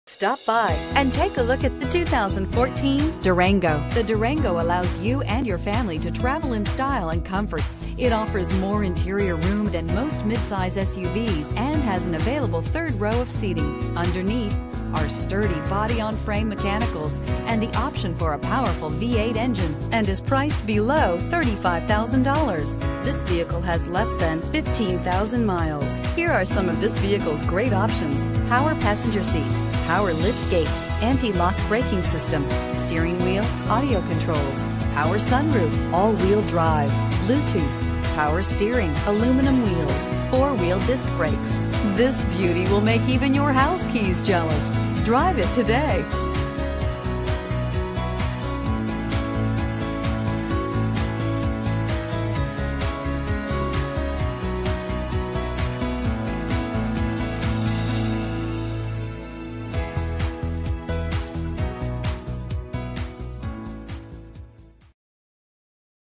2014 Dodge Durango 595462
2014 DODGE DURANGO Sterling, CO
Stock# 595462

Korf Continental Sterling
520 E. Chestnut Street

This clean car fax vehicle is in excellent condition, loaded with low miles. If a Durango RT is in your future then this vehicle is a must see. Call today for availability